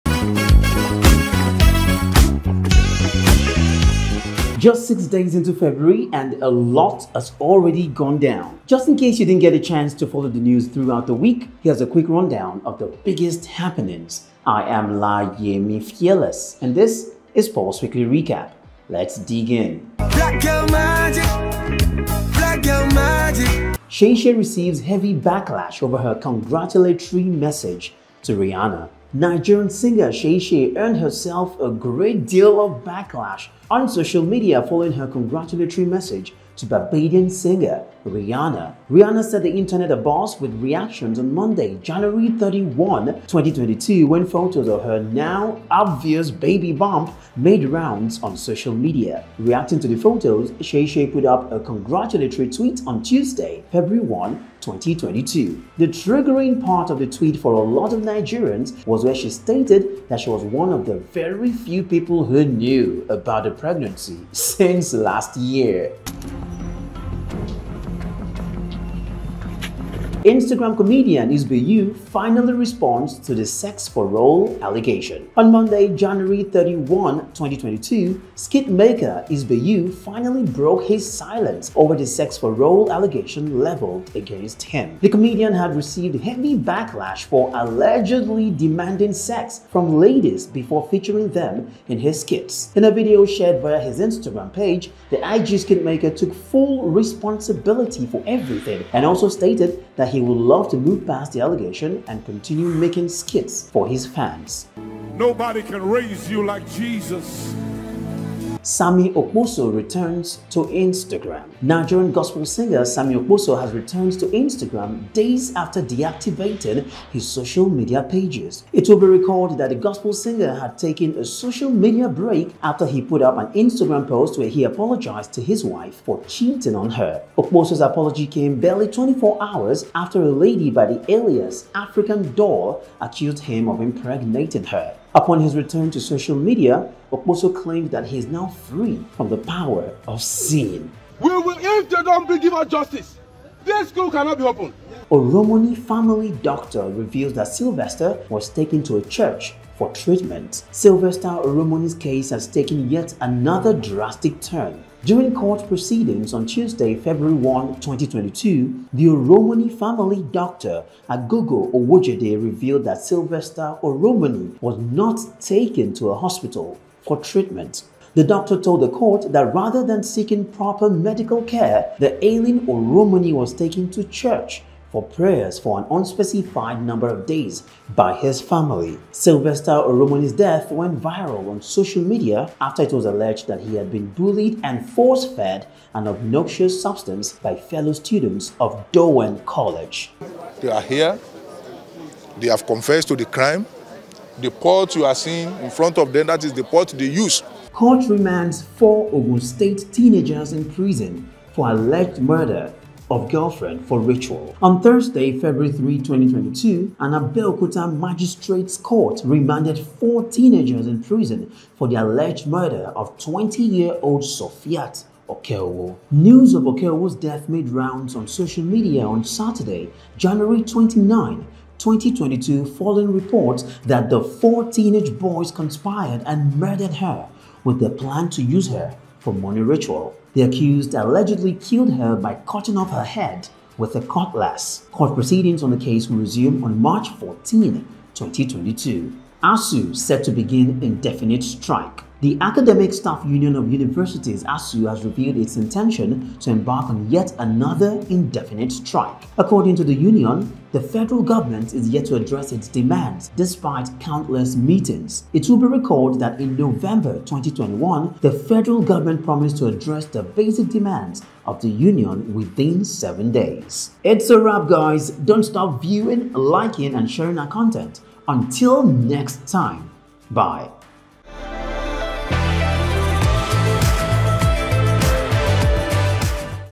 PULSE WEEKLY RECAP; SEYI SHAY RECEIVES HEAVY BACKLASH OVER HER CONGRATULATORY MESSAGE TO RIHANNA
Just six days into February and a lot has already gone down! Just in case you didn't get a chance to follow the news throughout the week, here's a quick rundown of the biggest happenings.

SEYI SHAY RECEIVES HEAVY BACKLASH OVER HER CONGRATULATORY MESSAGE TO RIHANNA

Nigerian singer, Seyi Shay, earned herself a great deal of backlash on social media following her congratulatory message to Barbadian singer, Rihanna.

Rihanna set the internet abuzz with reactions on Monday, January 31, 2022, when photos of her now obvious baby bump made rounds on social media.

Reacting to the photos, Seyi Shay put up a congratulatory tweet on Tuesday, February 1, 2022.

The triggering part of the tweet for a lot of Nigerians was where she stated that she was one of the very few people who knew about the pregnancy since last year.

INSTAGRAM COMEDIAN ISBAE U FINALLY RESPONDS TO THE SEX FOR ROLE ALLEGATION

On Monday, January 31, 2022, skit maker Isbae U finally broke his silence over the sex for role allegation levelled against him.

The comedian had received heavy backlash for allegedly demanding sex from ladies before featuring them in his skits.

In a video shared via his Instagram page, the IG skit maker took full responsibility for everything and also stated that he would love to move past the allegation and continue making skits for his fans.

SAMMIE OKPOSO RETURNS TO INSTAGRAM

Nigerian gospel singer Sammie Okposo has returned to Instagram, days after deactivating his social media pages.

It would be recalled that the gospel singer had taken a social media break after he put up an Instagram post where he apologised to his wife for cheating on her.

Okposo's apology came barely 24 hours after a lady by the alias African Doll accused him of impregnating her.

Upon his return to social media, Okposo claimed that he is now free from the power of sin.

OROMONI FAMILY DOCTOR REVEALS THAT SYLVESTER WAS TAKEN TO A CHURCH FOR TREATMENT

Sylvester Oromoni's case has taken yet another drastic turn.

During court proceedings on Tuesday, February 1, 2022, the Oromoni family doctor, Aghogho Owhojede revealed that Sylvester Oromoni was not taken to a hospital for treatment.

The doctor told the court that rather than seeking proper medical care, the ailing Oromoni was taken to church for prayers for an unspecified number of days by his family.

Sylvester Oromoni's death went viral on social media after it was alleged that he had been bullied and force-fed an obnoxious substance by fellow students of Dowen College.

COURT REMANDS 4 OGUN STATE TEENAGERS IN PRISON FOR ALLEGED MURDER OF GIRLFRIEND FOR RITUAL

On Thursday, February 3, 2022, an Abeokuta Magistrates’ Court remanded four teenagers in prison for the alleged murder of 20-year-old Sofiat Okeowo.

News of Okeowo's death made rounds on social media on Saturday, January 29, 2022, following reports that the four teenage boys conspired and murdered her with the plan to use her for money ritual.

The accused allegedly killed her by cutting off her head with a cutlass.

Court proceedings on the case will resume on March 14, 2022.

ASUU SET TO BEGIN INDEFINITE STRIKE

The Academic Staff Union of Universities (ASUU) has revealed its intention to embark on yet another indefinite strike.

According to the union, the Federal Government is yet to address its demands despite countless meetings.

It would be recalled that in November 2021, the FG promised to address the basic demands of the union within seven days.